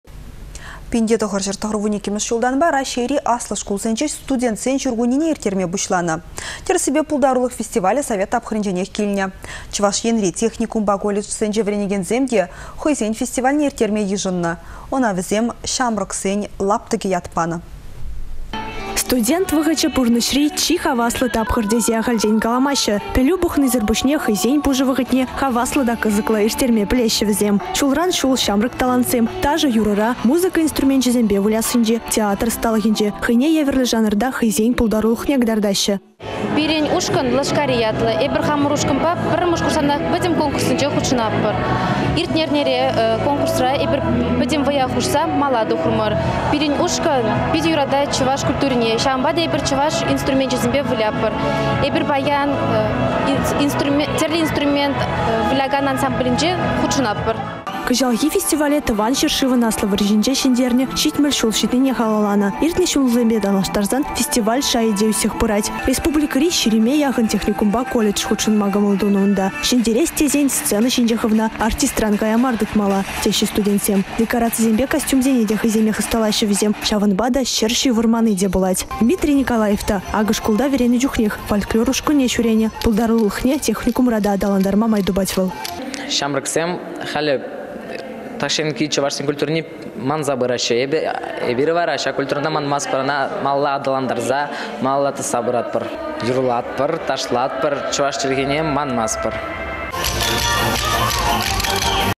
Çамрăксен лаптăкĕ 2015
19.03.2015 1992 çултанпа Раççейри Аслă шкулсенче студентсен çуркуннине ирттерме пуçланă. Тĕрĕссипе, пултарулăх фестивалĕ совет тапхăрĕнченех килнĕ. Чăваш Енри техникумпа колледжсенче вĕренекенсем те хăйсен фестивальне ирттерме йышăннă. Ăна вĕсем Çамрăксен лаптăкĕ ят панă.